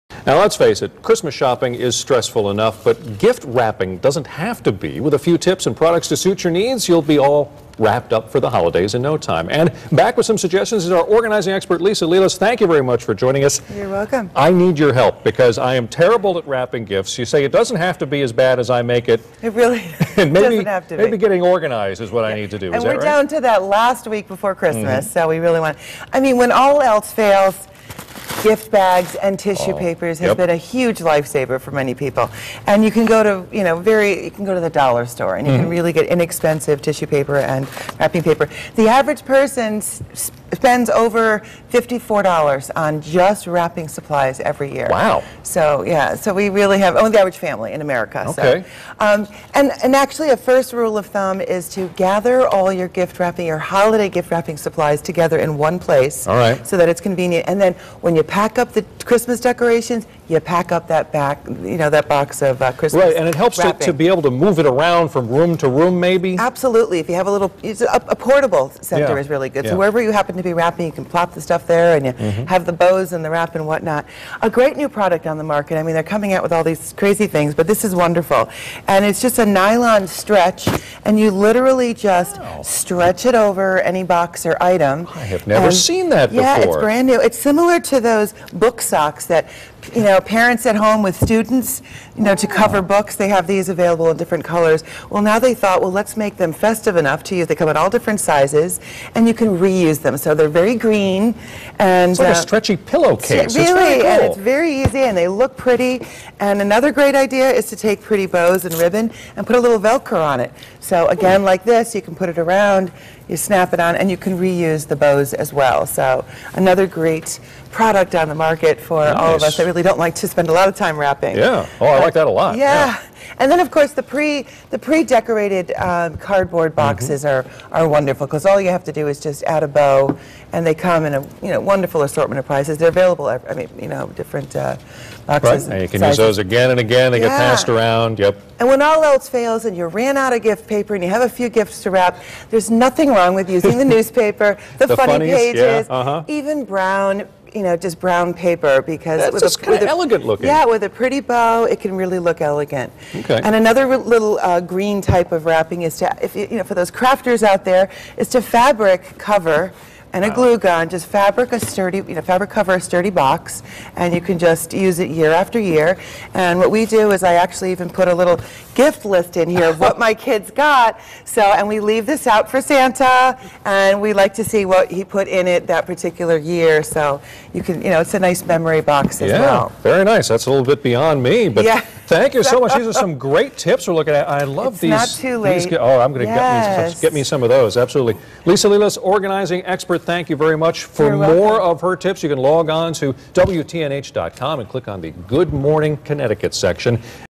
Wrapping it up for the holidays
Lets face it, Christmas shopping is stressful enough, but gift wrapping doesn't have to be: With a few tips and products to suit your needs you'll be all "wrapped up"  for the holidays in no time.